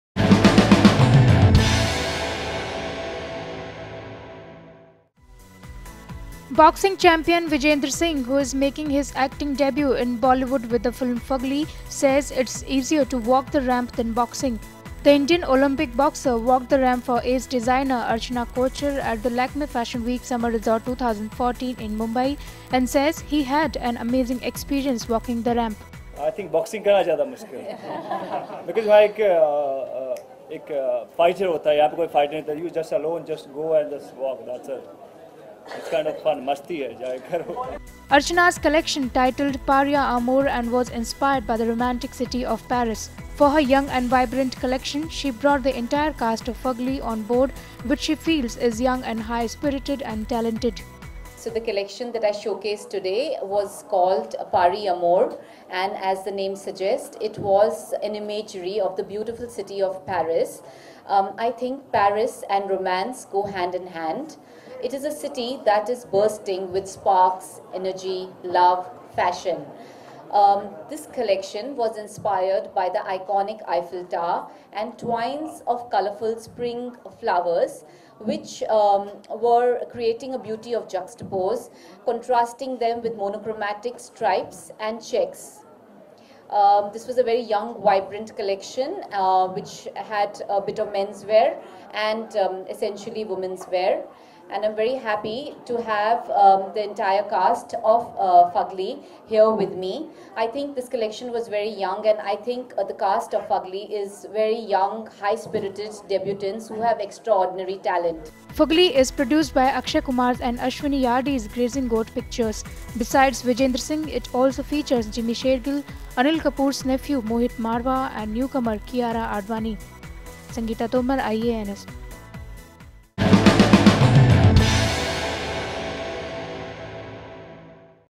Ramp-walk easier for boxer Vijender - IANS India Videos
Boxing champion Vijender Singh, who is making his acting debut in Bollywood with the film 'Fugly' says it's easier to walk the ramp than boxing.